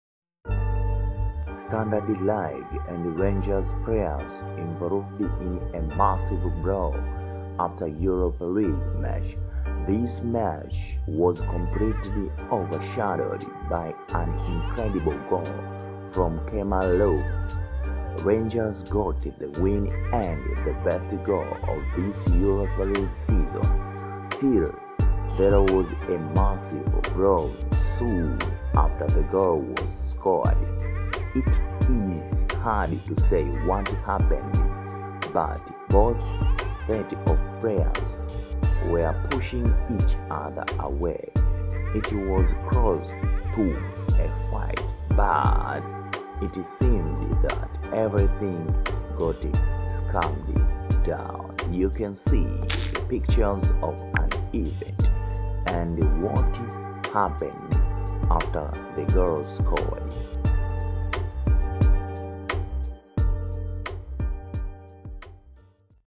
Standard Liege and Rangers angry incident 😠
Standard Liege and Rangers players in a massive brawl & pushing each other